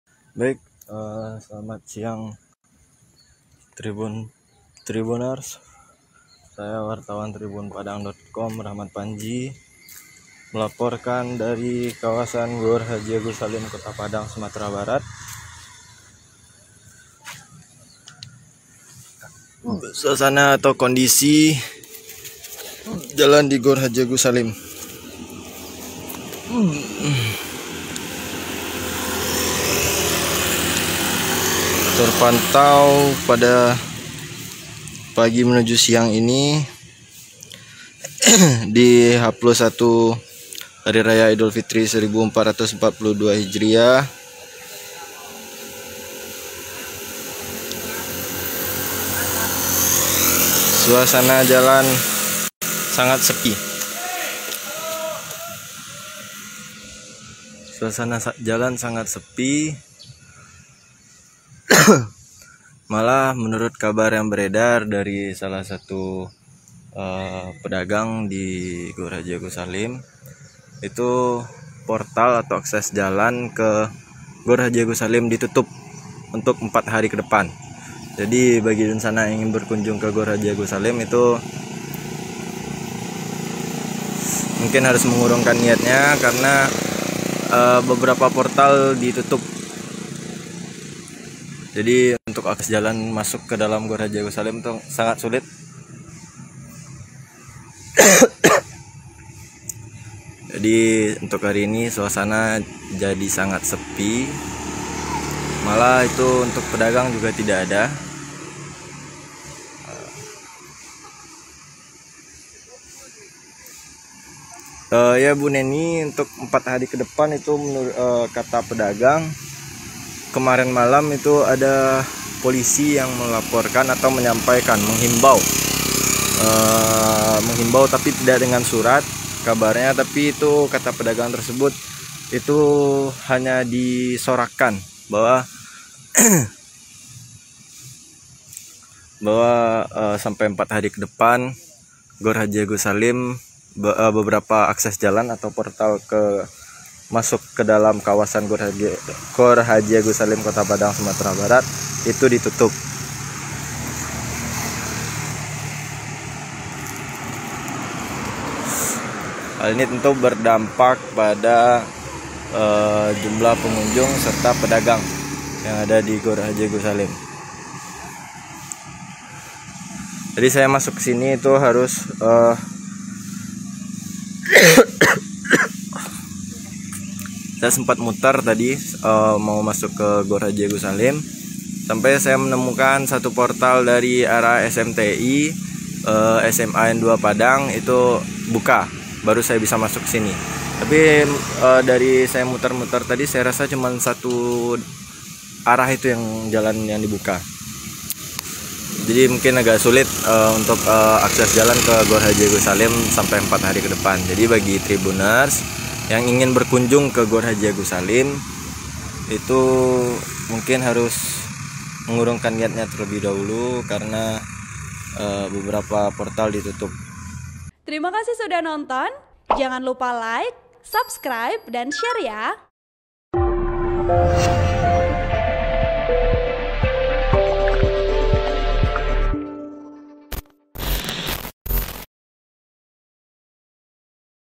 Kawasan GOR H Agus Salim Kota Padang Ditutup 4 Hari
TRIBUN-VIDEO.COM - Satu hari selepas hari raya Idul Fitri 1442 H kawasan GOR H Agus Salim, Kota Padang Sumatera Barat (Sumbar) ditutup.

Penutupan ini berlangsung mulai hari Jumat (14/5/2021) sampai 4 hari Ke depan.

Salah satu pedagang yang tidak mau disebutkan namanya mengatakan penutupan ini diumumkan oleh pihak kepolisian pada malam Kamis (13/5/2021).(*)